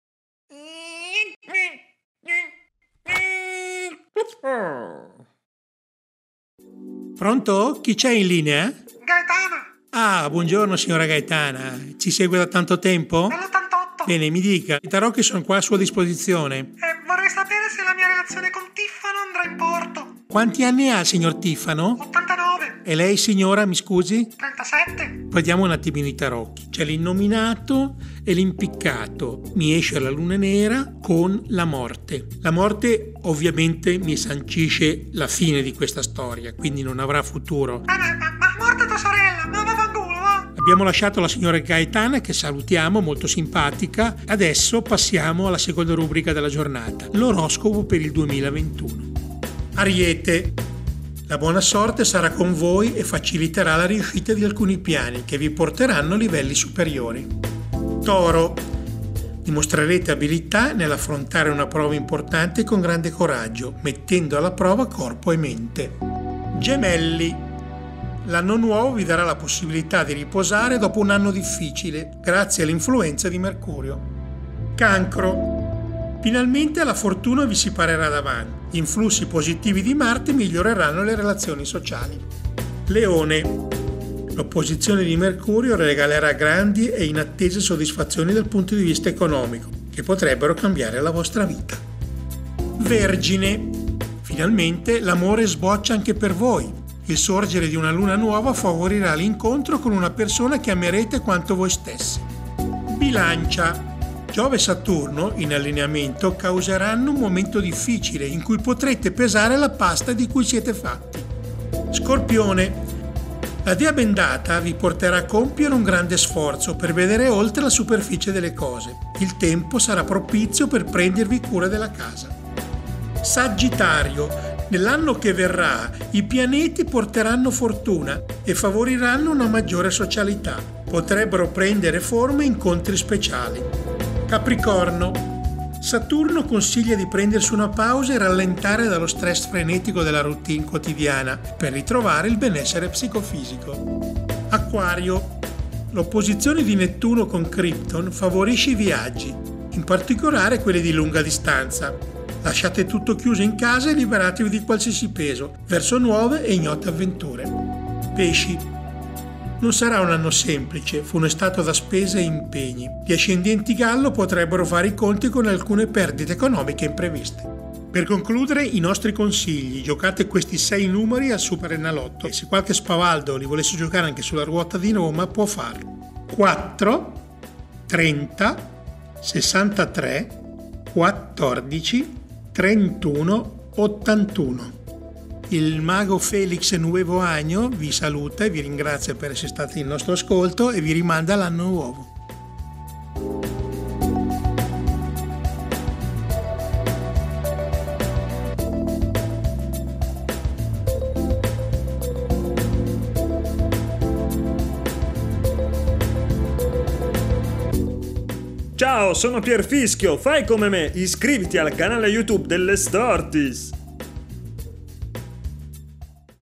Oroscopo 2021: le previstorties
Come sarà il 2021? Ve lo spiega il mago Feliz Nuoveaño grazie ai propri tarocchi: segno per segno, ecco le previsioni per l'anno che verrà illustrate dal nostro Pierfischio. E non perdete i consigli sui numeri da giocare al SuperEnalotto!